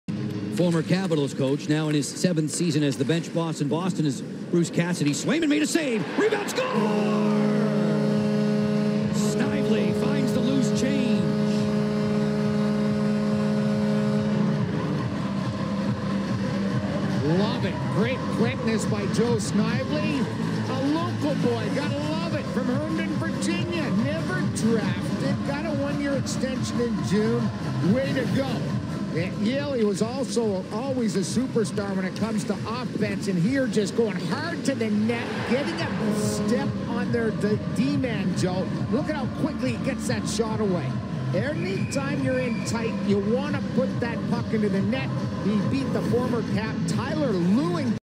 Joe Snively Opens The Scoring For Washington
The Caps take the early lead as Snively bangs home the loose puck as he falls to the ice making it 1-0 Washington.